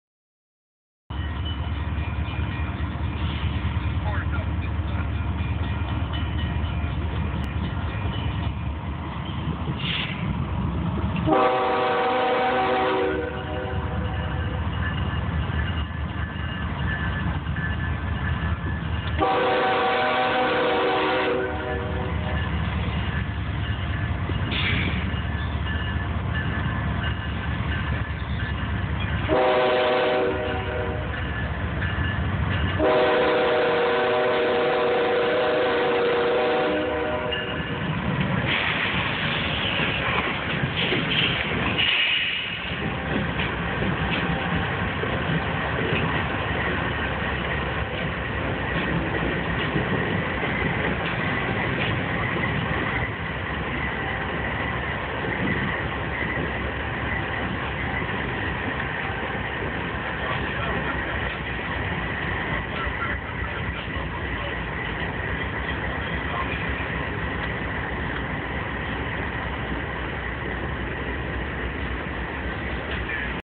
Ns 24e
Ns 24e rolling in to Norris Yard irondale with evo on point and Oli dash9 3rd